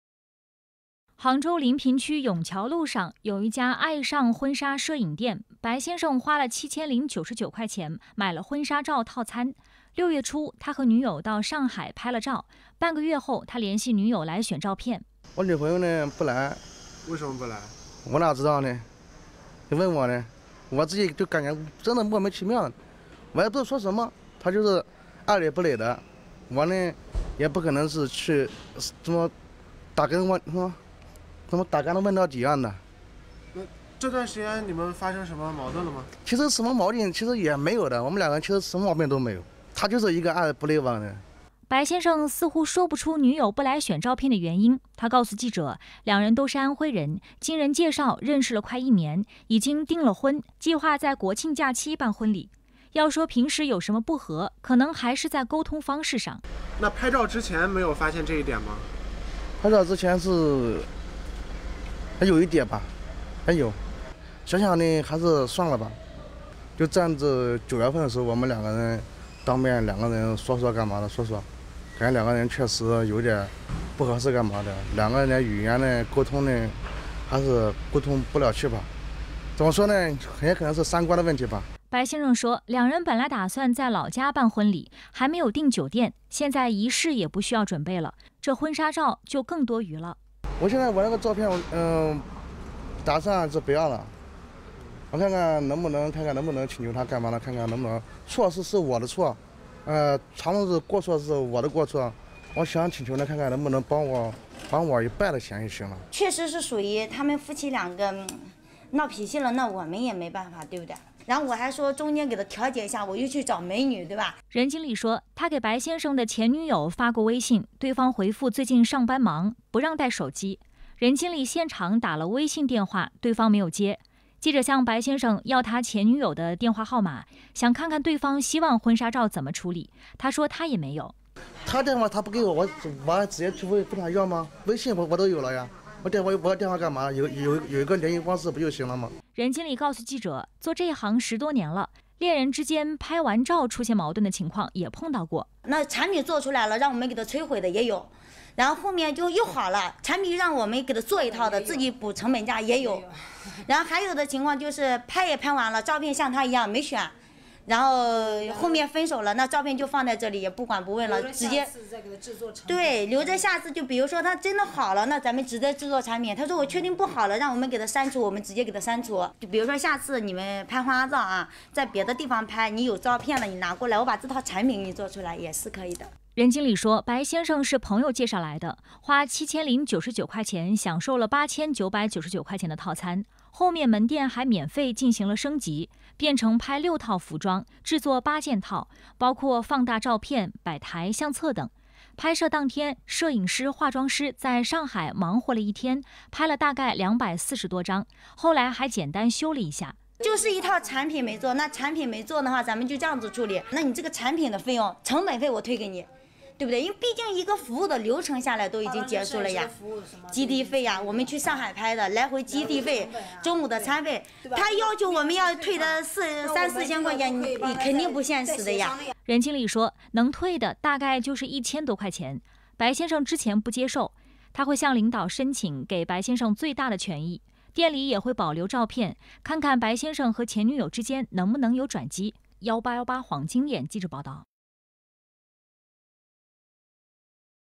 【1818黄金眼】拍婚纱照后女友变前女友，门店希望有转机
【拍婚纱照后女友变前女友  门店希望有转机】白先生跟女友拍了婚纱照。还没等到选照片，女友就成了前女友。照片现在不打算要了，他希望摄影店能够退一半费用。记者想问问女方的意思，白先生说，前女友的电话他也没有。婚纱店这边说，类似的情况他们倒是见多了，有些后来又和好了，又要照片了。白先生退费这事要再向公司申请，照片店里会保留，万一有转机呢。

新闻标签：